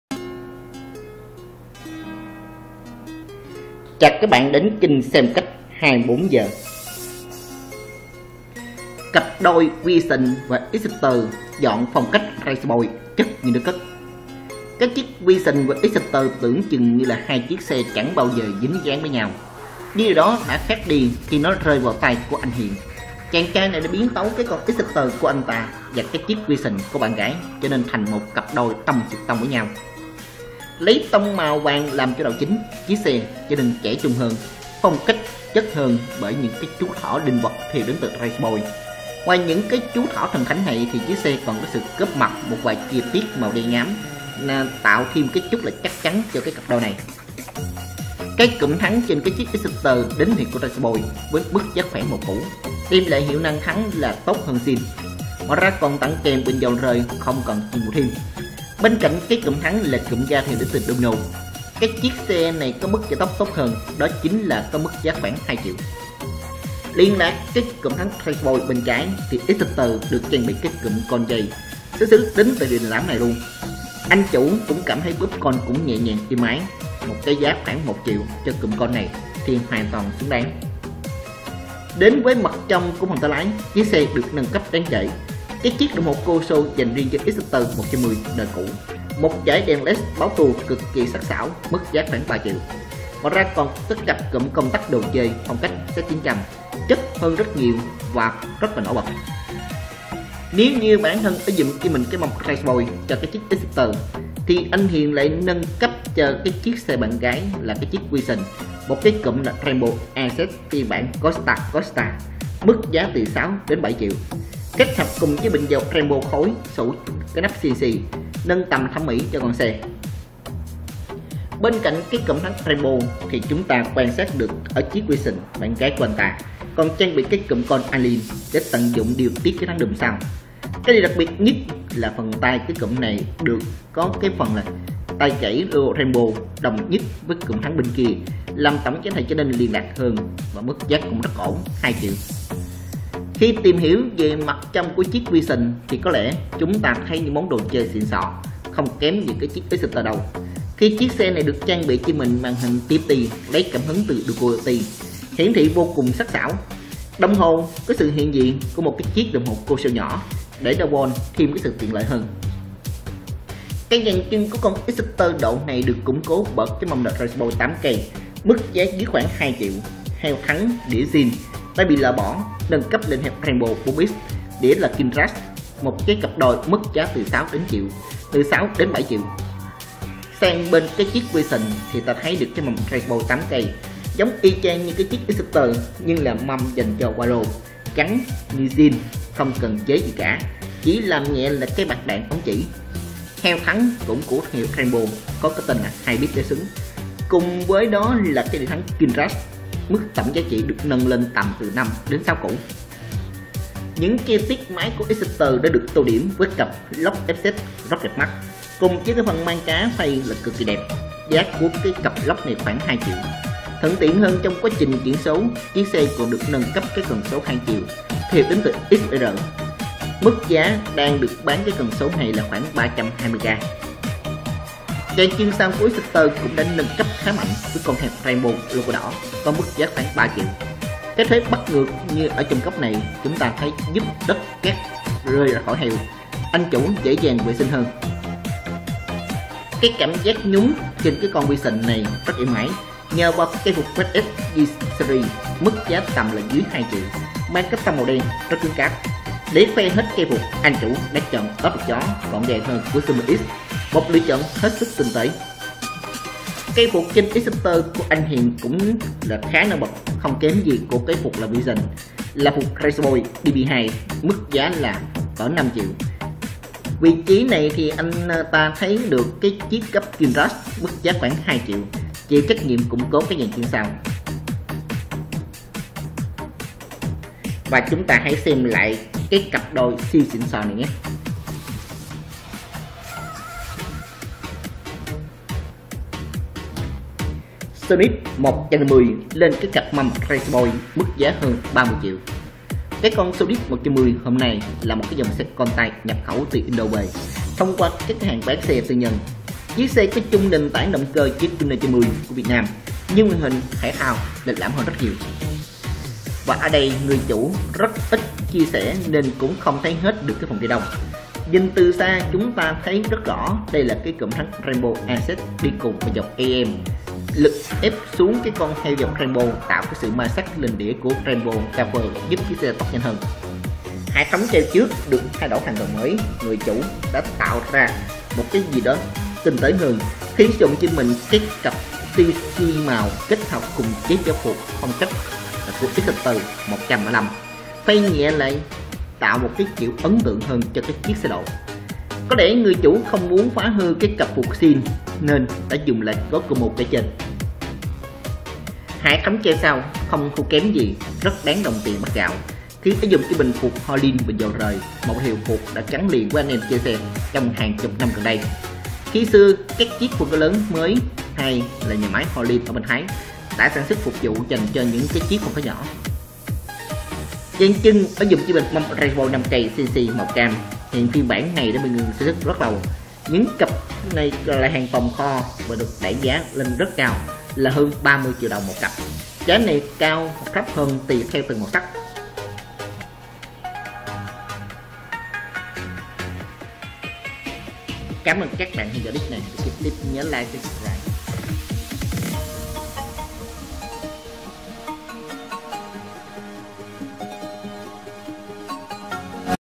Xe & Phong Cách 24h - Cặp đôi Vision và Exciter dọn phong cách RCB chất hơn nước cất.
Vision và Exciter tưởng chừng là hai chiếc xe chẳng bao giờ dính dáng với nhau, nhưng điều đó đã khác khi nó rơi vào tay anh Hiền. Anh chàng đã biến tấu chiếc Exciter của anh và Vision của bạn gái để trở thành một cặp đôi tông xuyệt tông với nhau.